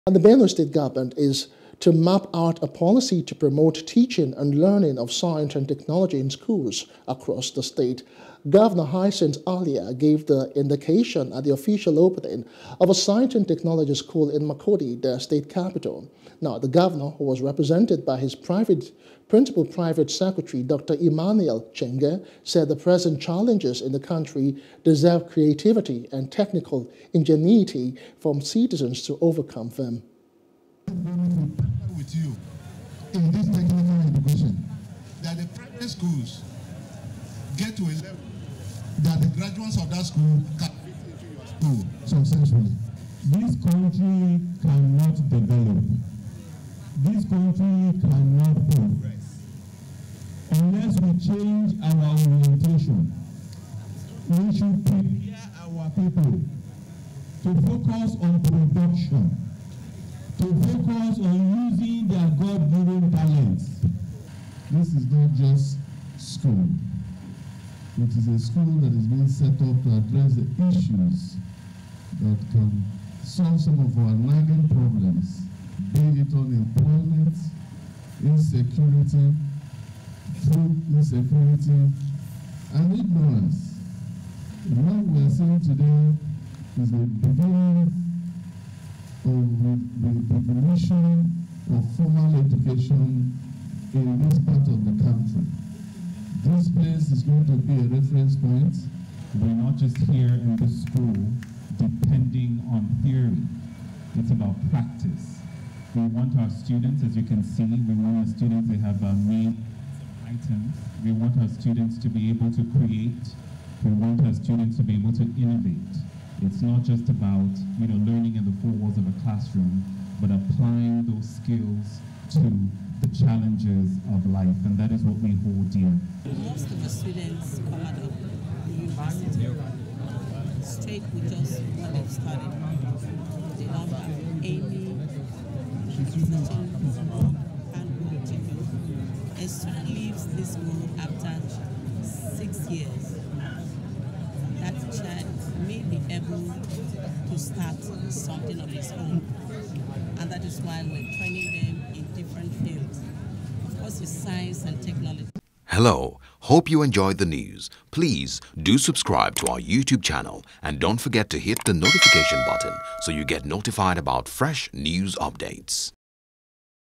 Benue Government Partners Private Sector On Science Education.
The Benue State Government is to map out a policy to promote teaching and learning of science and technology in schools across the State.